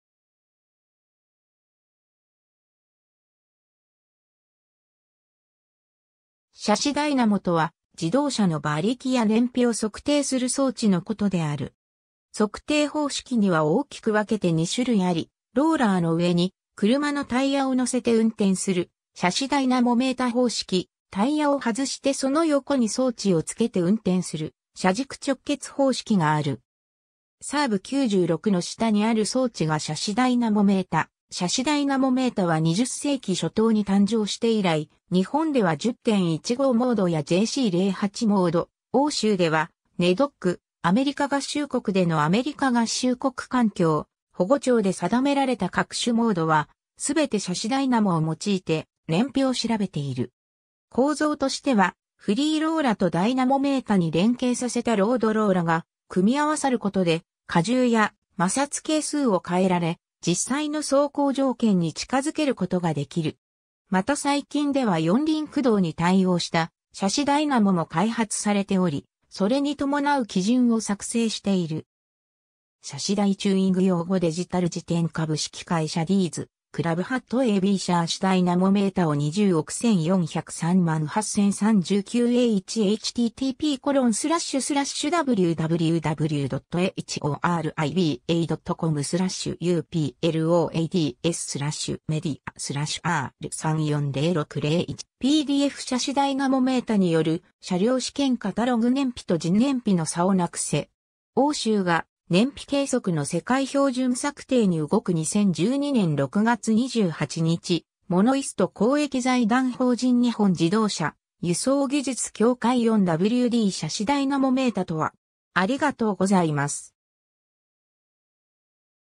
シャシダイナモ
シャシダイナモ（英語: Chassis dynamometer）とは、自動車の馬力や燃費を測定する装置の事である。
 測定方式には大きく分けて2種類あり、ローラーの上に車のタイヤを載せて運転するシャシダイナモメータ方式、タイヤを外してその横に装置をつけて運転する車軸直結方式がある。
 サーブ・96の下にある装置がシャシダイナモメータ シャシダイナモメータは20世紀初頭に誕生して以来、日本では10・15モードやJC08モード、欧州ではNEDC、アメリカ合衆国でのアメリカ合衆国環境保護庁で定められた各種モードは、すべてシャシダイナモを用いて燃費を調べている。
 構造としてはフリーローラとダイナモメータに連携させたロードローラが組み合わさる事で、荷重や摩擦係数を変えられ、実際の走行条件に近づけることが出来る。
また最近では四輪駆動に対応したシャシダイナモも開発されており、それに伴う基準を作成している。
^ シャシダイ チューイング用語デジタル辞典 株式会社 ディーズ・クラブ ^ a b シャーシダイナモメータ WO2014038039 A1 ^ シャシダイナモメータによる車両試験 (PDF) ^ カタログ燃費と実燃費の差をなくせ！欧州が燃費計測の世界標準策定に動く 2012年06月28日 MONOist ^ 公益財団法人 日本自動車輸送技術協会 4WDシャシダイナモメータとは（役割と機能）